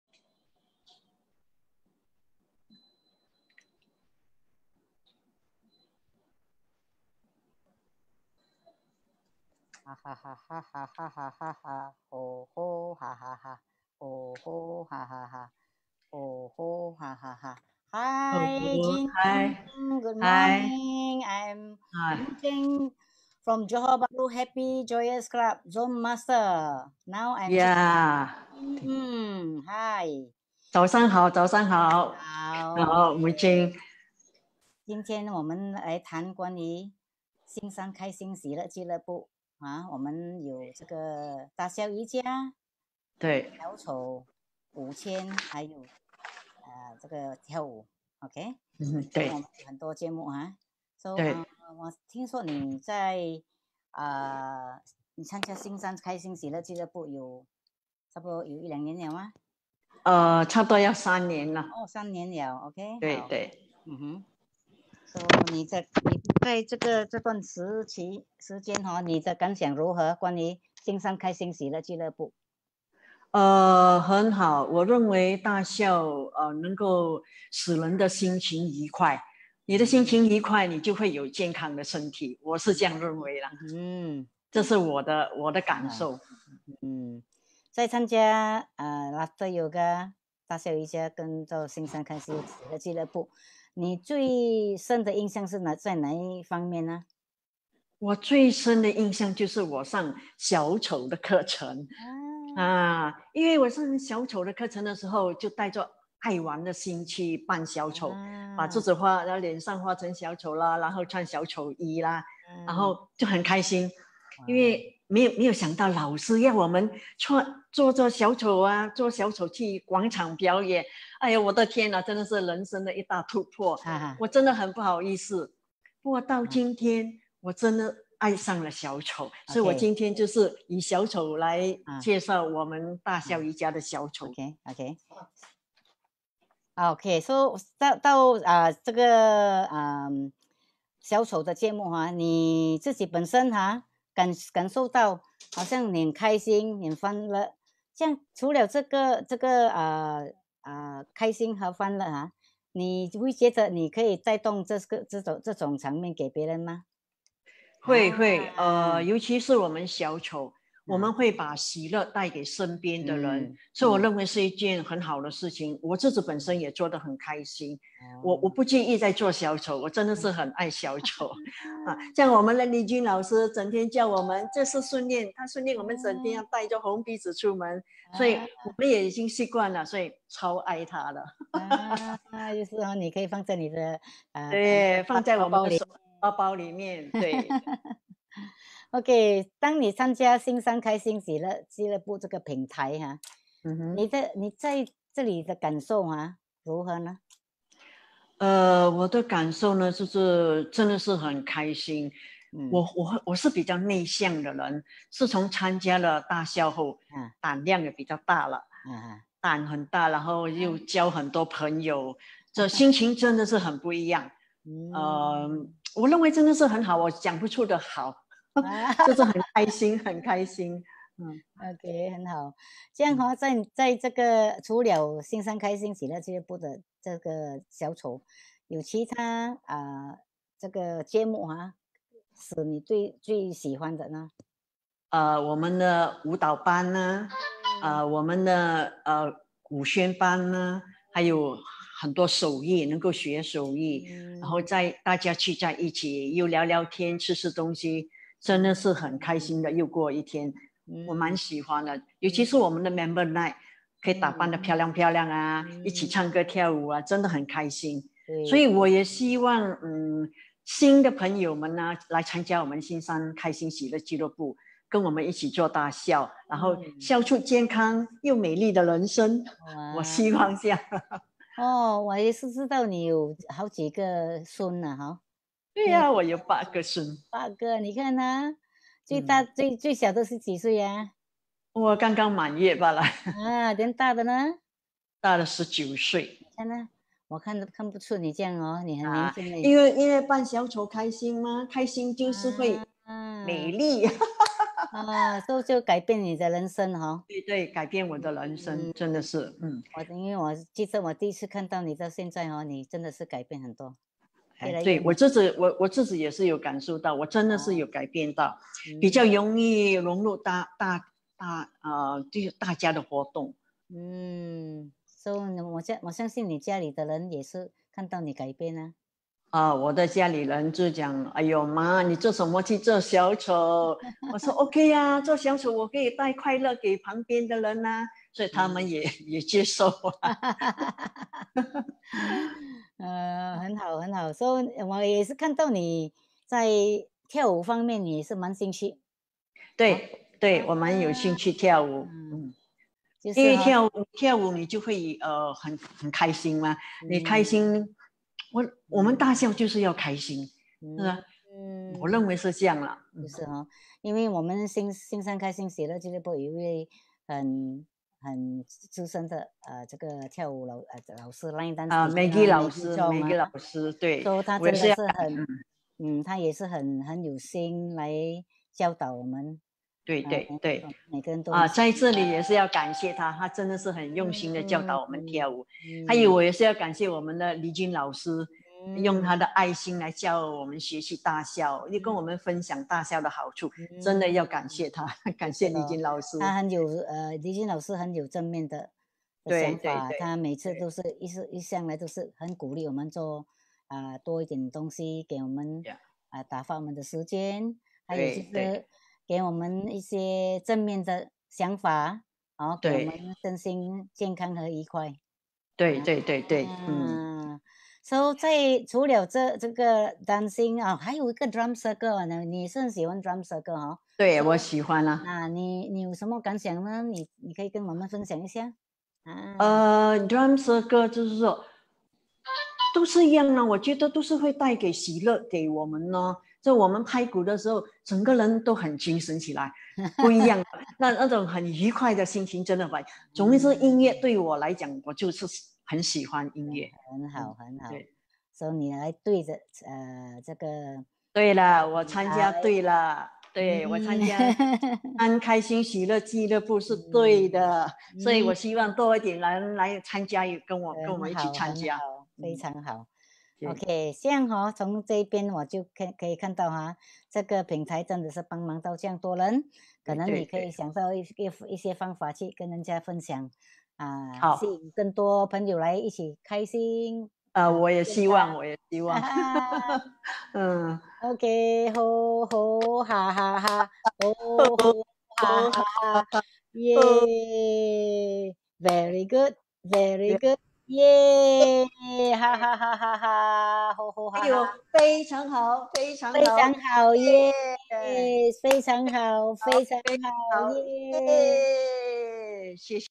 Chitchat No. 33 between Mui Cheng & Jean Ng on Laughter Yoga & Clowning
13th July 2020
Chitchat No. 33 between Mui Cheng & Jean Ng. Clowning and Laughter Yoga, activities of Johor Bahru Happy and Joyous Club